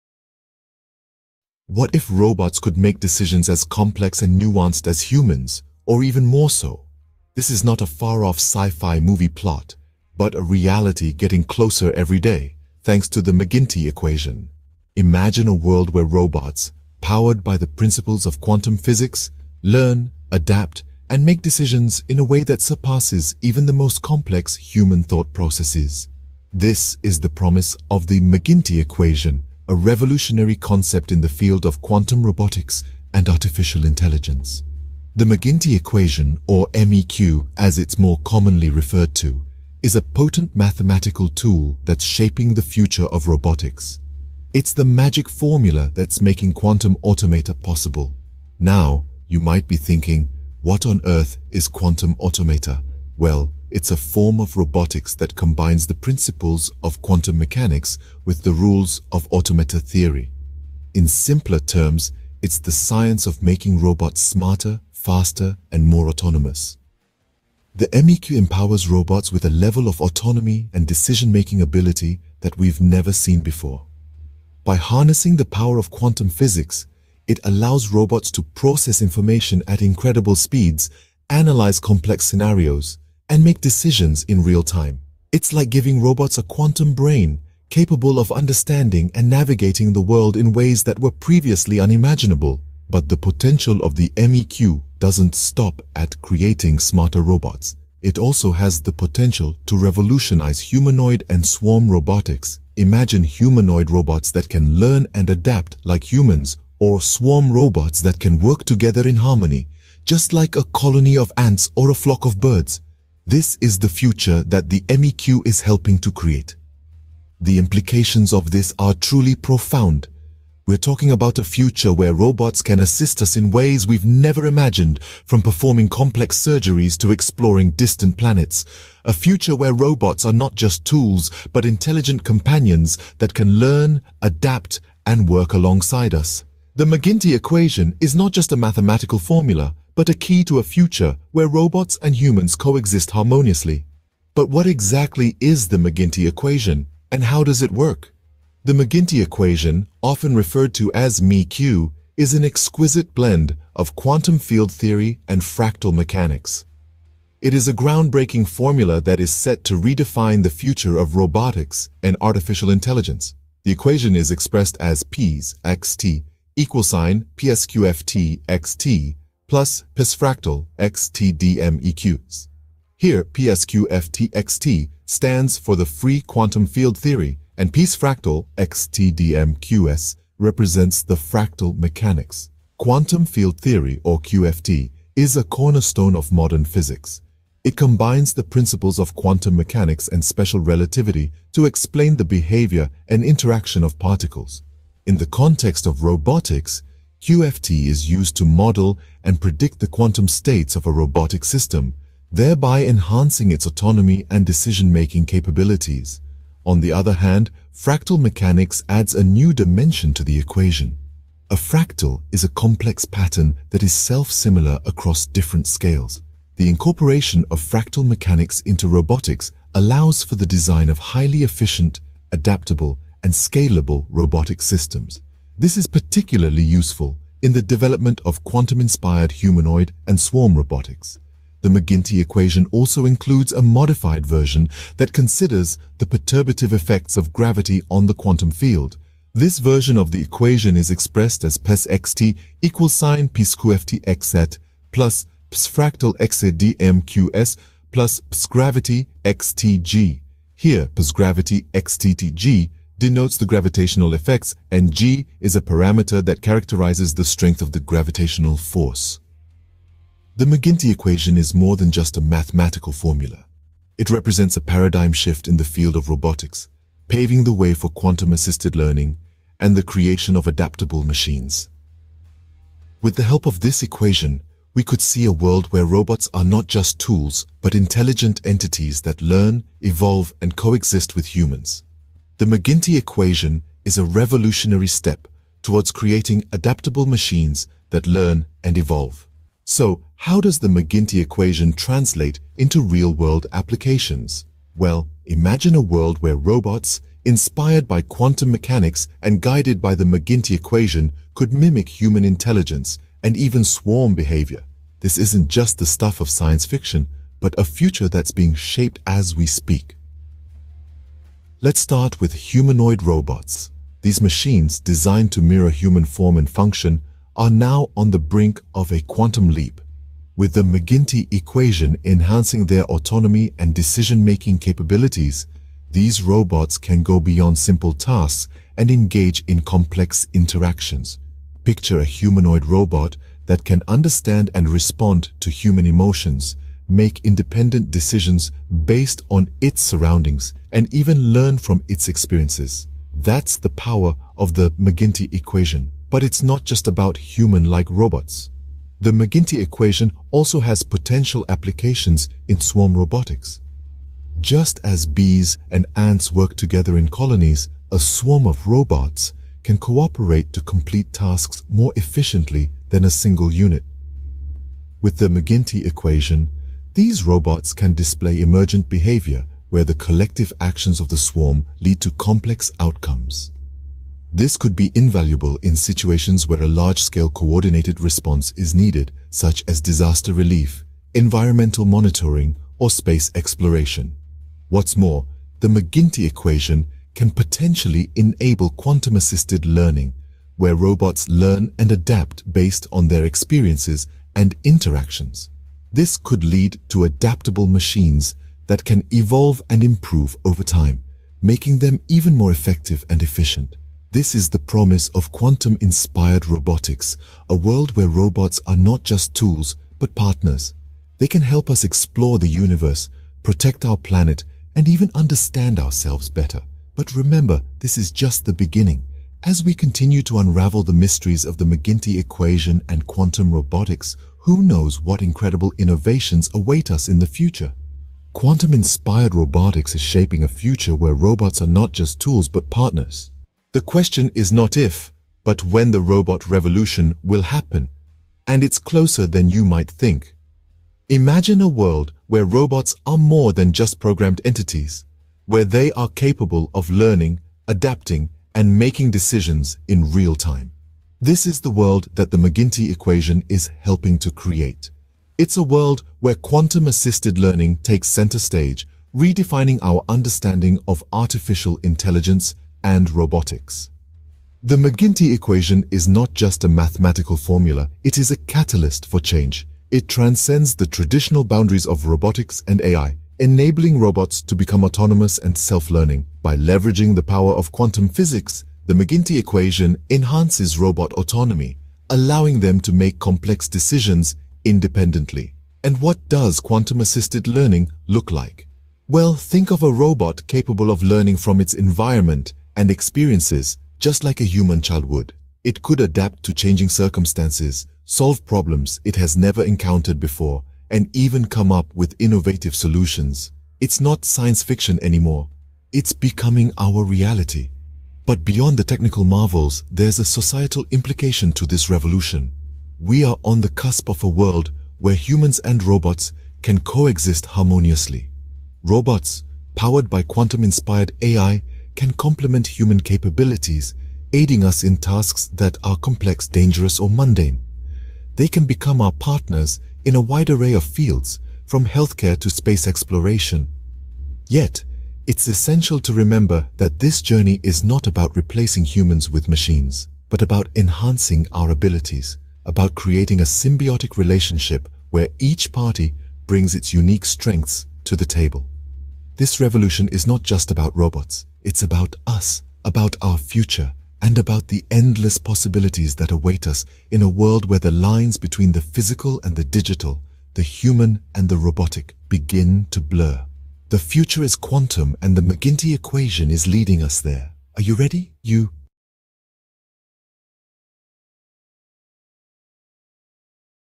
Quantum Leap: The McGinty Equation (MEQ) & the Future of Robotics
Welcome to "Quantum Leap: The McGinty Equation (MEQ) & the Future of Robotics"! In this captivating video, we delve into the cutting-edge world of robotics and its exciting intersection with the groundbreaking MEQ.

Join us as we unlock the immense potential of the Unified Equation of Physics (MEQ) in revolutionizing robotics. Discover how the MEQ's quantum-inspired insights are reshaping the design, intelligence, and capabilities of robots, propelling them into a new era of innovation.

Explore how quantum computing and MEQ-driven algorithms are empowering robots with unparalleled computational abilities, enabling them to process vast amounts of data and make decisions in real-time, fostering a truly autonomous and adaptive generation of robotic systems.

Witness astonishing advancements in quantum sensor technology, allowing robots to perceive their surroundings with unparalleled accuracy and sensitivity, making them more perceptive, agile, and capable of navigating complex environments.

From robotic exoskeletons that empower humans with superhuman strength to AI-powered drones that autonomously map and explore uncharted territories, this video showcases the transformative impact of the MEQ on the future of robotics.

Whether you're a tech enthusiast, a robotics expert, or simply curious about the boundless potential of quantum-driven automation, this video offers an awe-inspiring glimpse into the incredible world of Quantum Leap: The McGinty Equation (MEQ) & the Future of Robotics.

With the MEQ as our guiding force, we are unlocking the true potential of robotic technology, pushing the boundaries of what's possible. Don't miss this thrilling exploration of Quantum Leap: The McGinty Equation (MEQ) & the Future of Robotics!